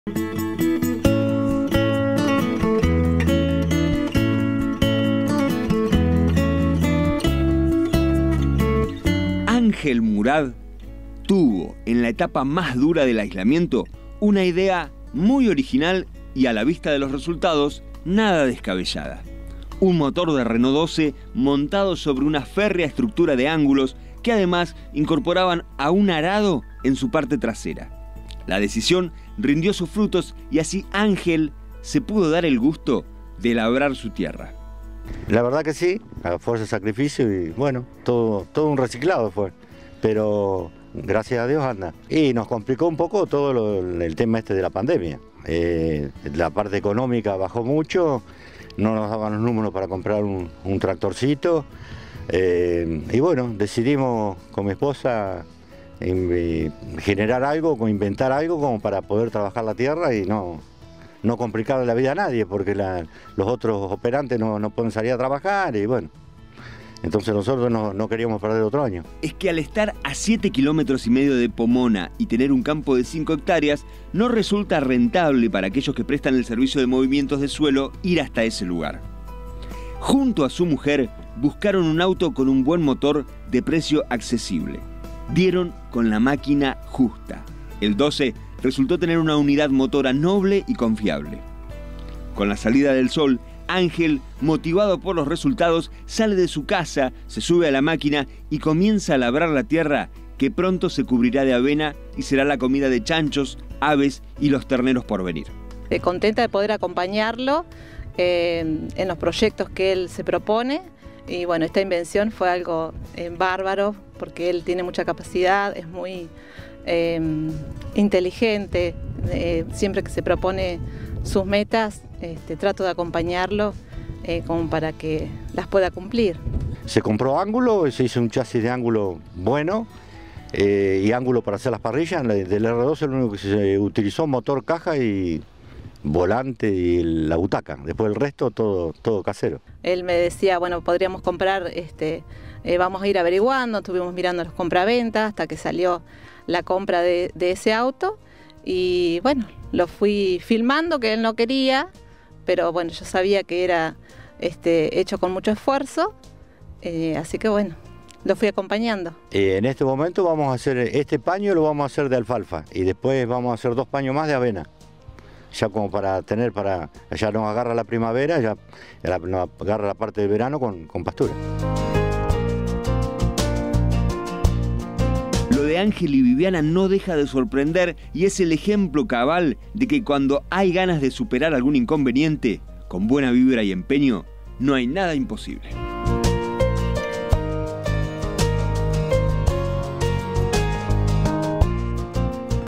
Notircias10 | Transformó lo que quedaba de un R12 en una máquina de arado
Un productor del Valle Medio de Río Negro no contaba con los fondos para comprar un tractor, pero su ingenio no conoce de límites.